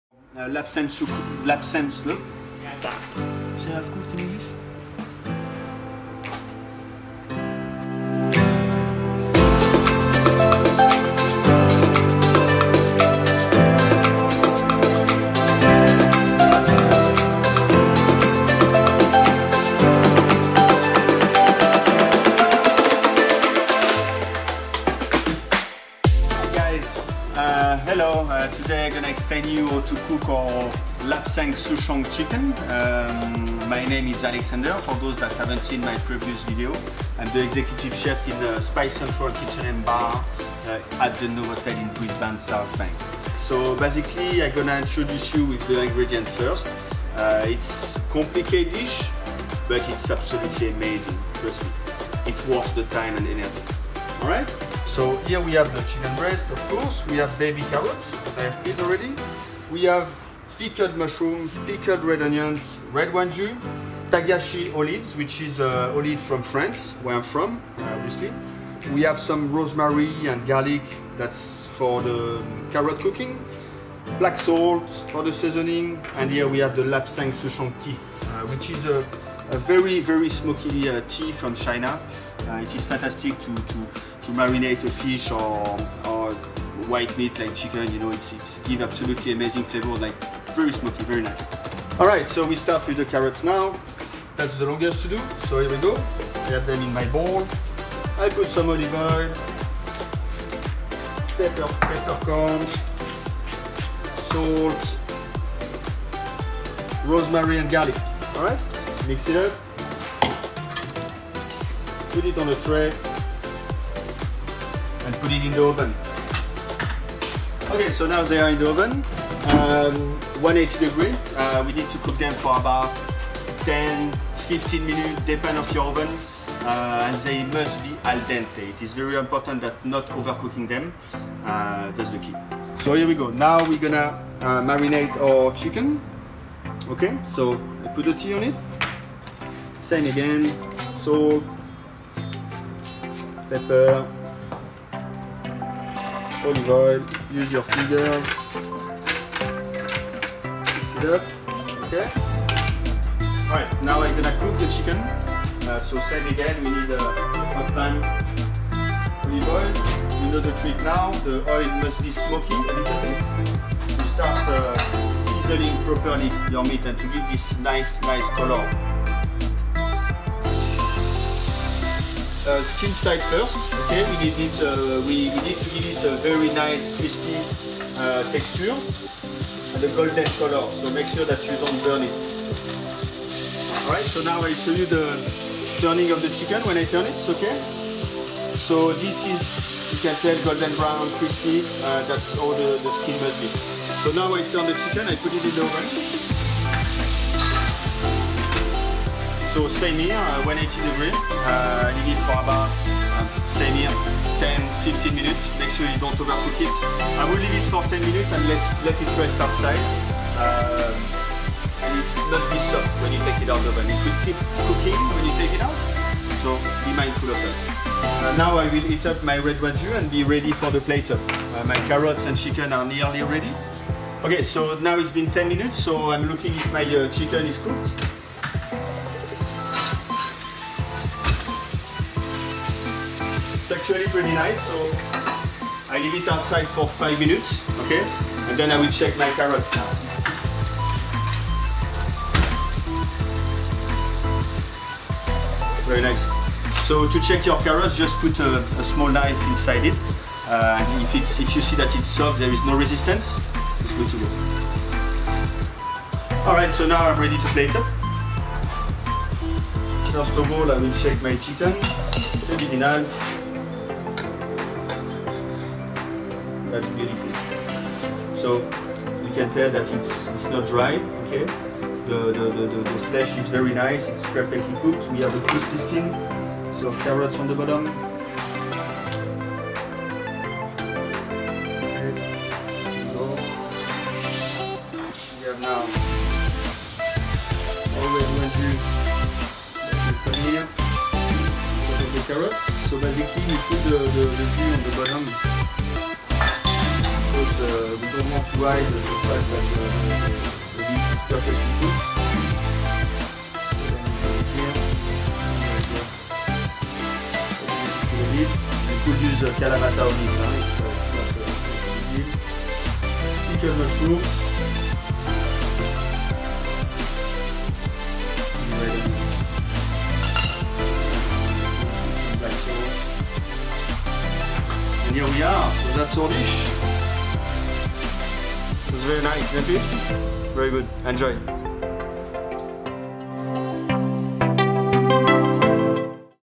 We infuse this chicken with Lapsang Souchong tea, the results are unbelievable.
This Instagram worthy chicken dish is as delicious as it looks.

See the menus or book a table at Spice Central Kitchen & Bar now:

38 Cordelia St
South Brisbane QLD 4101

Ingredients of dish:
Chicken breast     1
Lime       ½
Salt       15g
Taggiasche olives   1Tbls
Dutch carrots    5
pickled mushrooms                     6
lapsang souchong tea                 30ml
Sliced Red onion     ¼
Yuzu juice     10ml
Olive oil      10ml
Salt       10g
Peppercorn     2g
Red wine jus (1Liter of jus)
Beef bones     1 kg
Red wine      1.5L
Tomato paste     75g
Carrot      2
Onion      1
Leak      1
Garlic      2 cloves
Butter      50g